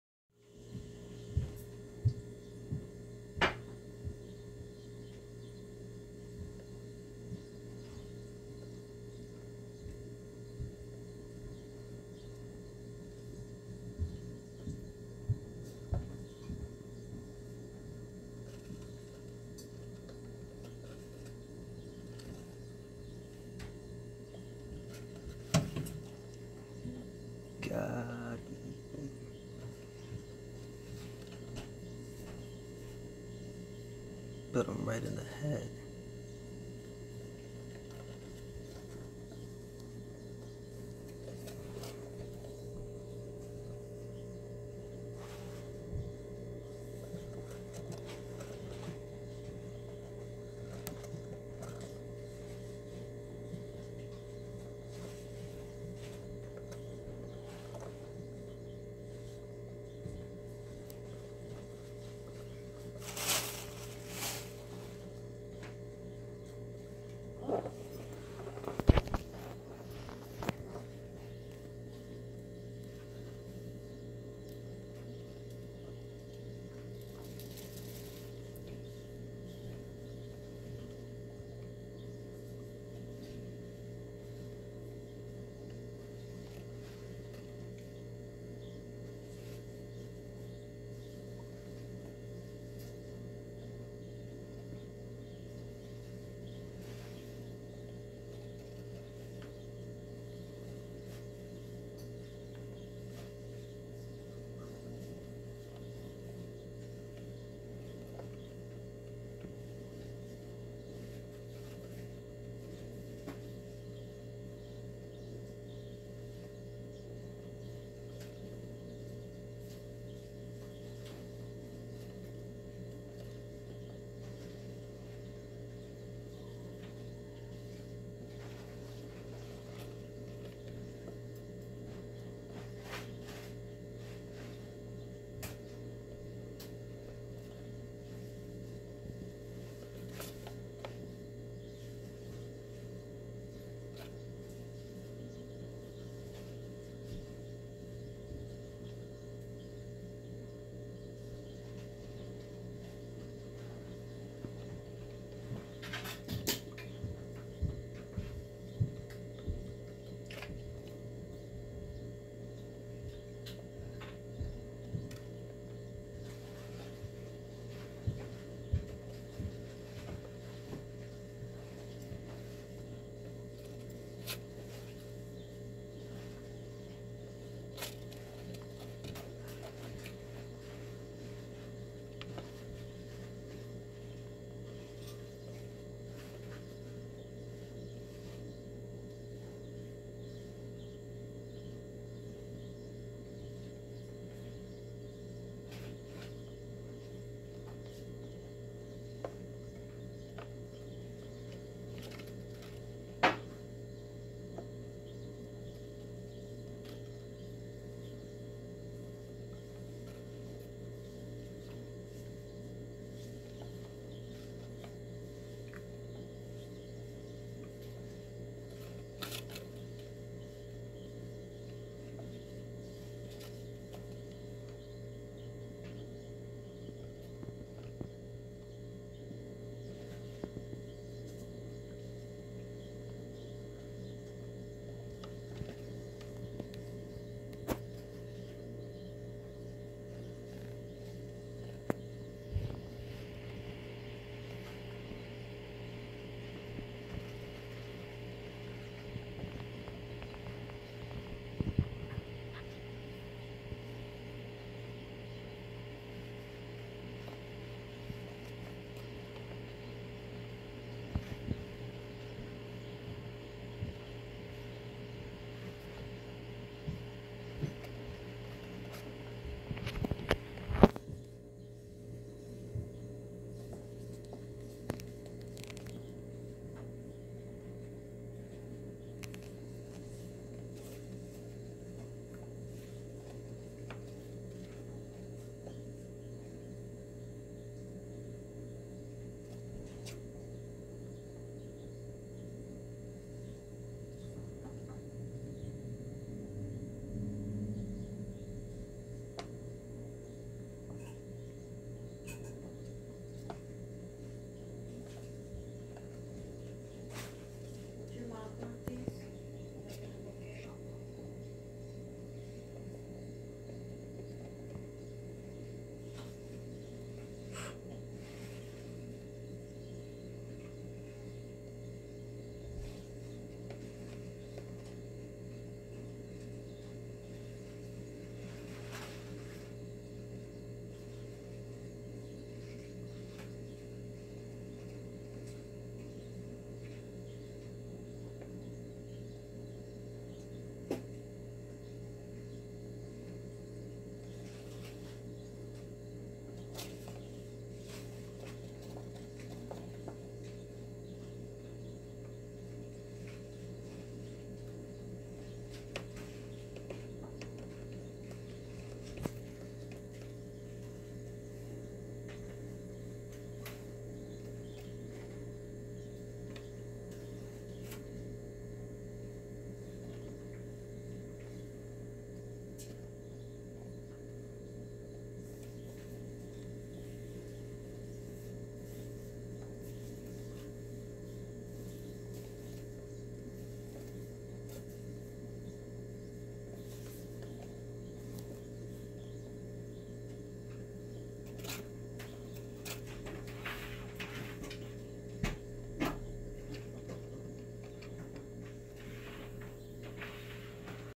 Suriname true red tail boa VS mouse
Here is Zoey my True Red Tail Boa, she's a BCC and a very good eater. Never ever refused a meal..